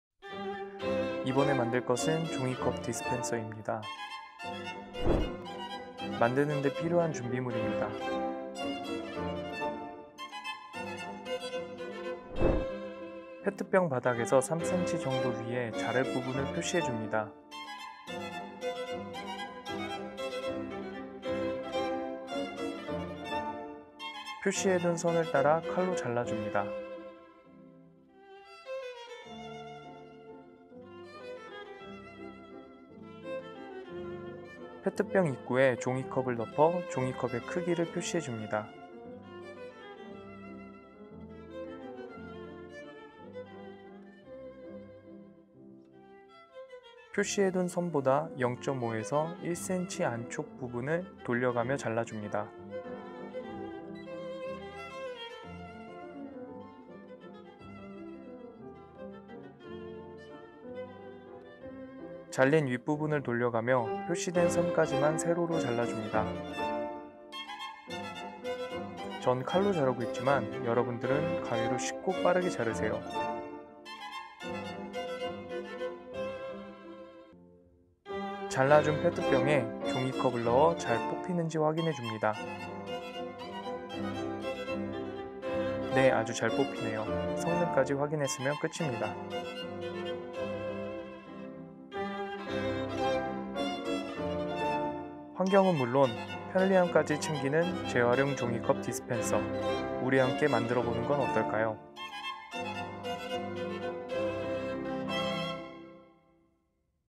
재활용품의 무한변신 '종이컵 디스펜서 만들기'
준비물 – 페트병, 종이컵, 칼, 펜

- 페트병의 바닥에서 3cm 정도 위에 자를 부분을 표시해 줍니다.
      표시해둔 선을 따라 칼로 잘라 줍니다.

- 페트병 입구에 종이컵을 덮어 종이컵의 크기를 표시해 줍니다.
 표시해둔 선보다 0.5~1cm 안쪽 부분을 돌려가며 잘라줍니다.

- 잘린 윗부분을 돌려가며 표시된 선까지만 세로로 잘라 줍니다.
 전 칼로 자르고 있지만, 여러분들은 가위로 쉽고 빠르게 자르세요

- 잘라준 페트병에 종이컵을 넣어 잘 뽑히는지 확인해 줍니다.
     네. 아주 잘 뽑히네요. 성능까지 확인했으면 끝입니다.

- 환경은 물론 편리함까지 챙기는 재활용 종이컵 디스펜서
 우리 함께 만들어 보는 건 어떨까요?